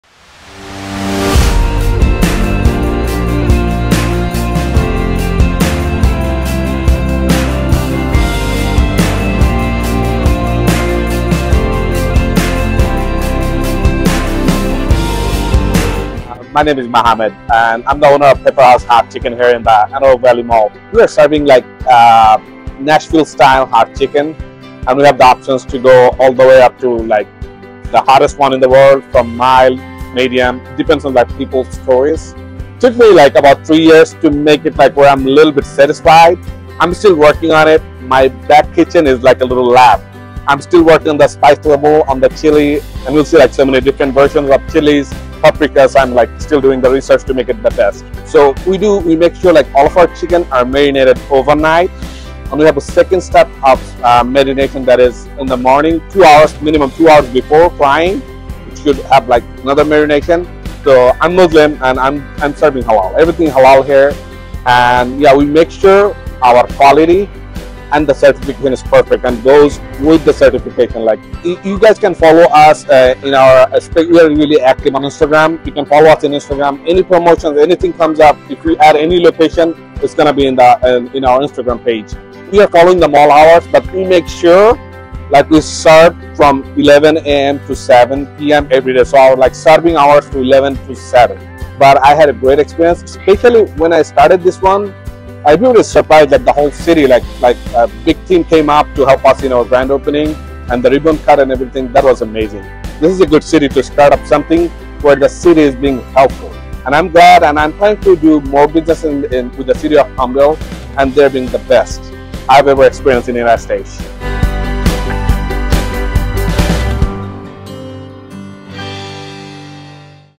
Business of the Week - Pepper House Hot Chicken
Check out the Highlight Video for Palmdale's Business of the Week: Pepper House Hot Chicken!

Recently opened at the AV Mall food court, they offer a Halal menu and serve hot chicken sandwiches and wings. Five different spice levels available from mild to x-treme if you dare.

Open Mon - Sat 11 am - 7 pm, Sun 11 am - 6 pm.